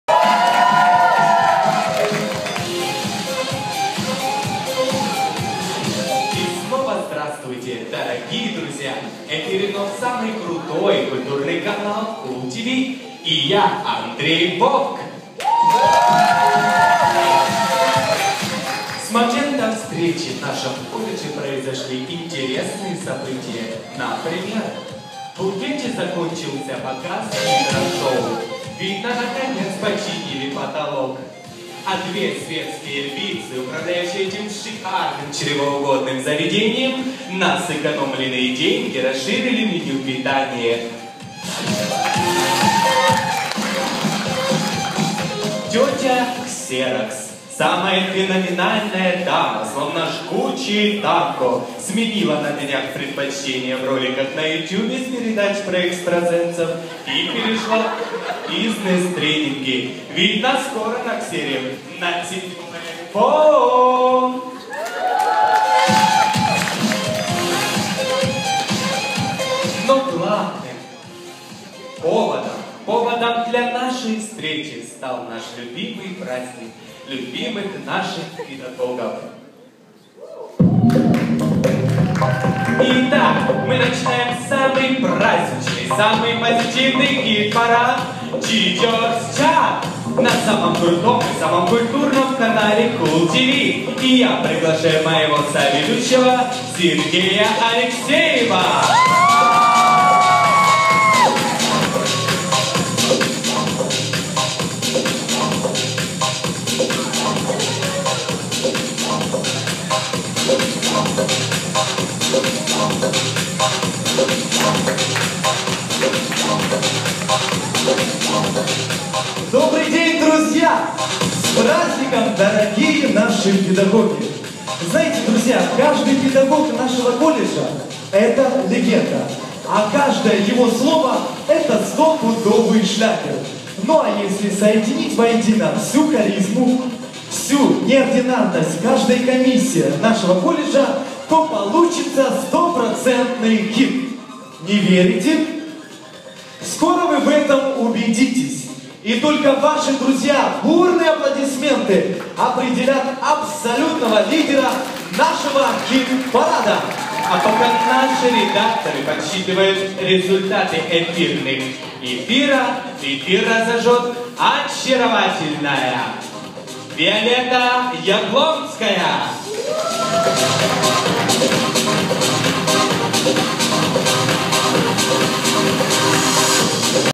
Ведущие Андрей Вовк и Сергей Алексеев
День Учителя Днепропетровский колледж культуры и искусств 2016